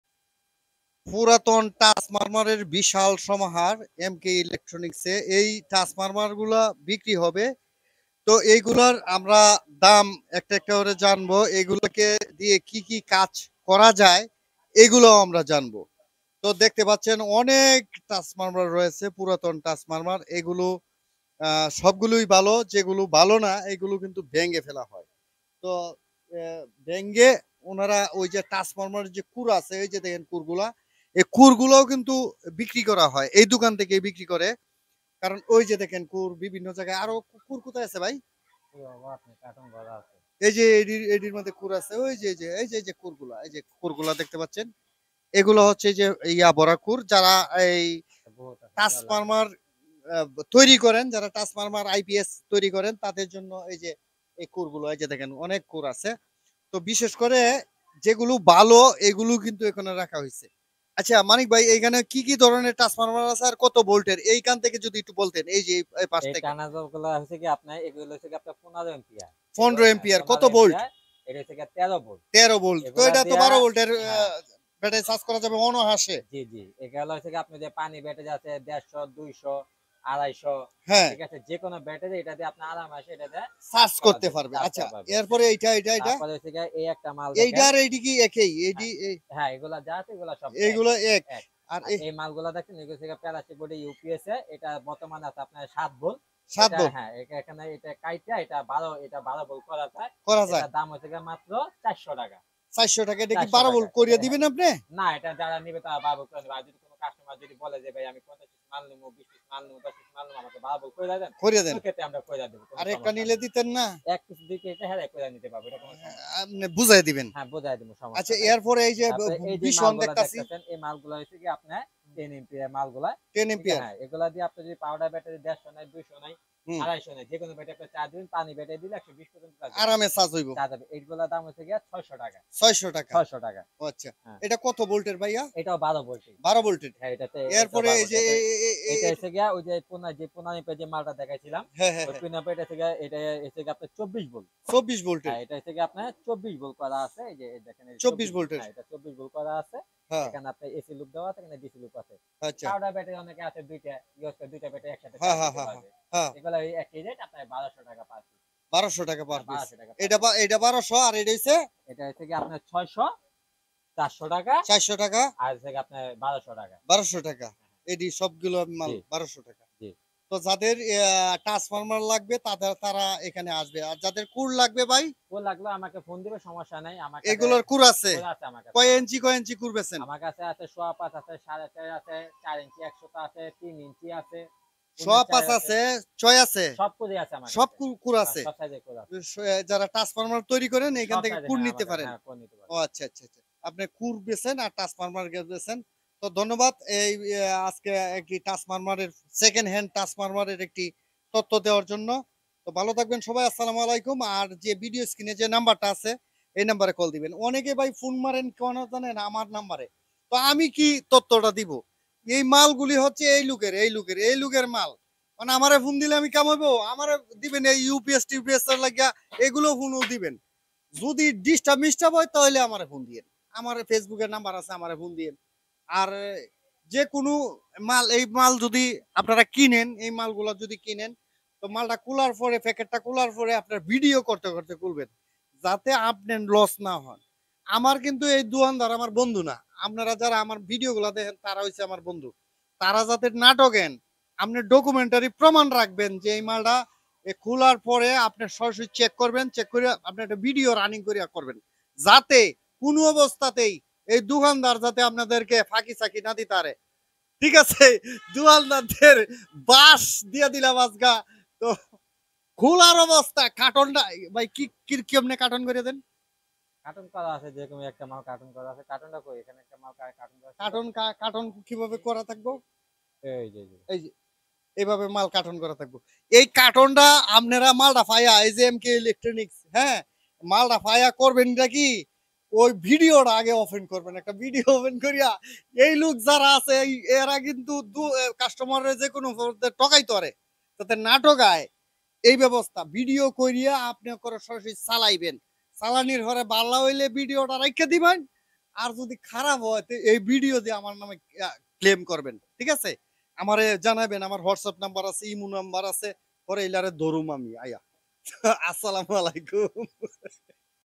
সেকেন্ড হ্যান্ড ট্রান্সফরমারের দাম | second hand transformer price | 10 /15 ampere Transformer price
এখানে যে সব ধরনের ট্রান্সফরমার পাওয়া যায়, তার মধ্যে উল্লেখযোগ্য হচ্ছে ইউপিএস এর পুরাতন ট্রান্সফরমার, ৮ টেম্পিয়ার ১০ এম্পিয়ার ও ১৫ এম্পিয়ার।
তাছাড়া ট্রান্সফারমারের বিভিন্ন ধরনের কোর বিক্রি করা হয়, নিকটস্থ মালগুলো যোগাযোগ করতে অবশ্যই নিচে যোগাযোগের নাম্বার দেওয়া হলো।

পুরাতন ইউপিএস বিক্রি করা হয়।
এম কে ইলেক্ট্রনিক বাসন সড়ক গাজীপুর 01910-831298

যেকোনো পণ্যের বিজ্ঞাপন দিতে যোগাযোগ করুন